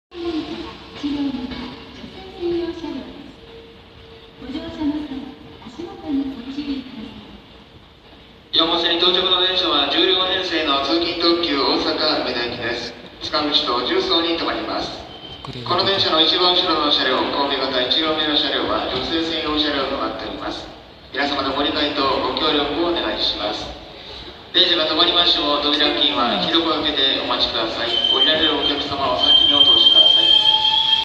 阪急7000系7037F通勤特急大阪梅田行き @西宮北口駅[HK08]入線
収録:阪急神戸線･西宮北口駅[HK08]
2019年12月19日撮影
車両:阪急7000系7037F(未更新車)
チョッパー
種別･行先:通勤特急 大阪梅田行き
8時46分入線